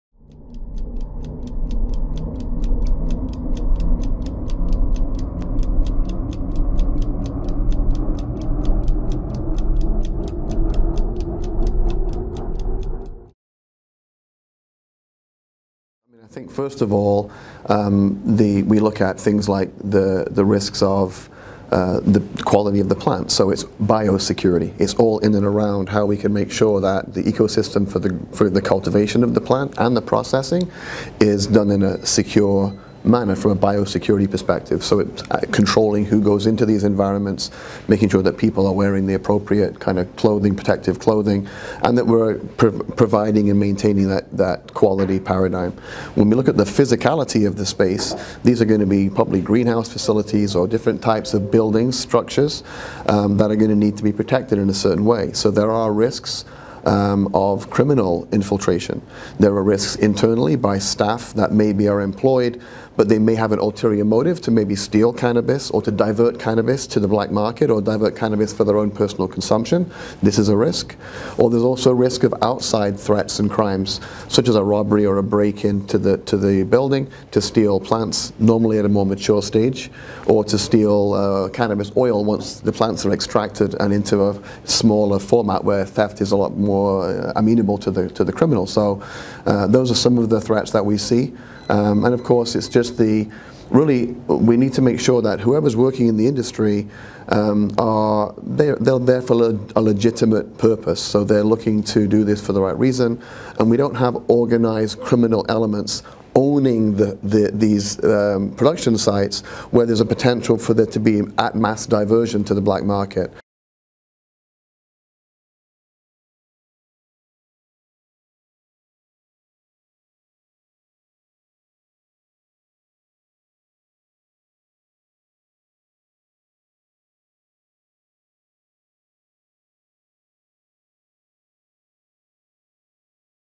What is Bio Security?
What is bio security? Find out with us from David Hyde. Learn more in the 4th episode of Stoned Age! 🌿🌴